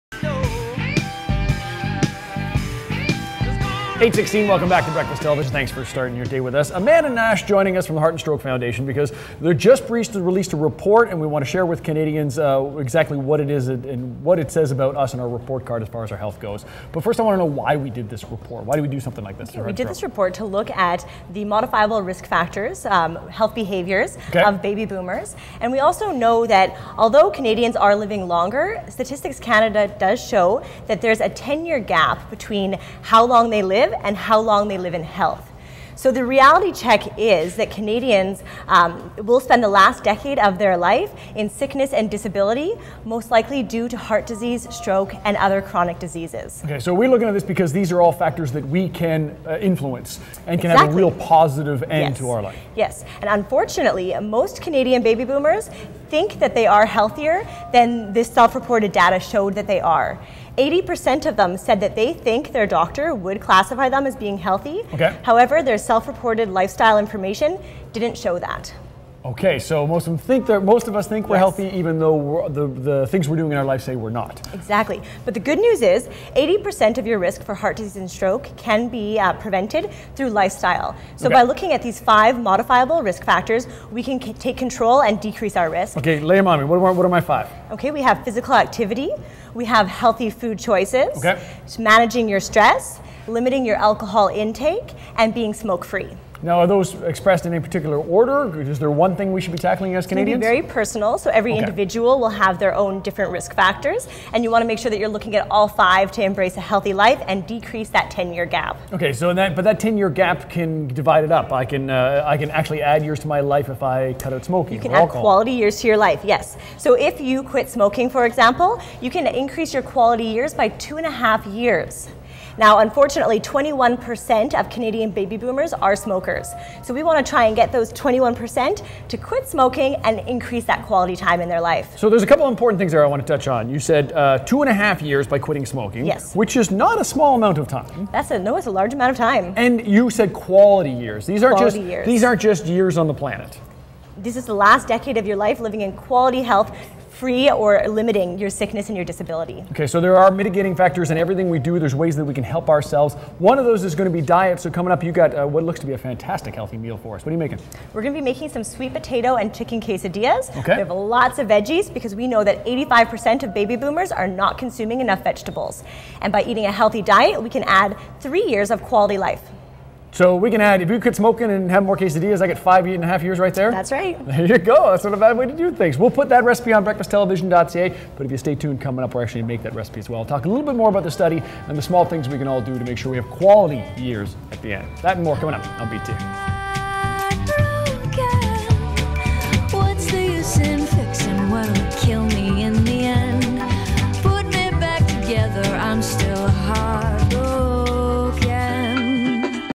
Heart & Stroke Foundation Report
RD Amanda Nash discusses heart health